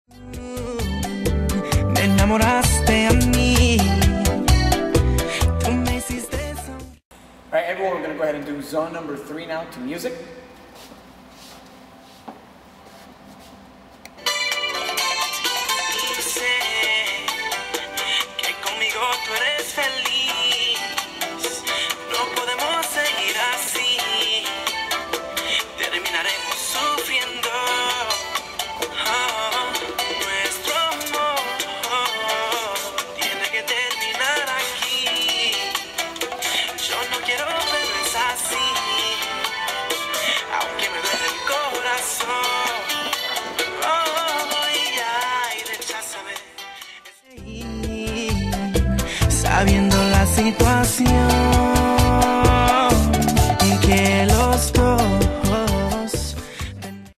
2nd International Bachata Flashmob 2012 ( Organized by Bahhari ) ZONE 3 to Music
Christian Sola Director of Bahhari & Sensual Bachata presents the 2nd International Bachata Flashmob.  We were able to accomplish the 1st ever International Bachata Flashmob in October 2011 with the participation of Atlanta & Florida from the US, and Hungary Debrecen from Europe.  This year we have the participation from many other states as well as many more countries from Europe and we are excited to see the results.  Here's the Intro to all the instructions to follow and what to expect.